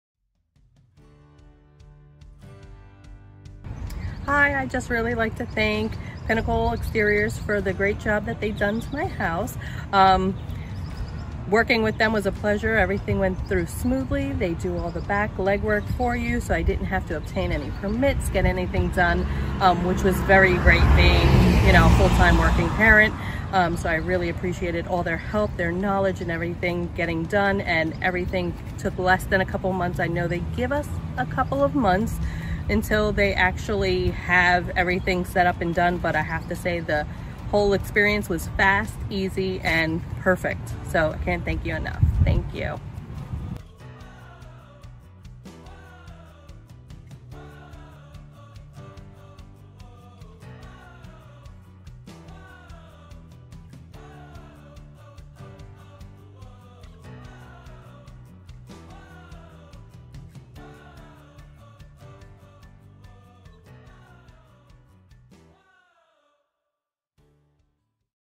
Melissa's Solar Testimonial Done in Denver, PA
Melissa and her husband live in a single-family home in Denver, PA where solar panels were installed through Pinnacle Exteriors. Through her testimonial, Melissa shares her feedback on the company and the overall process.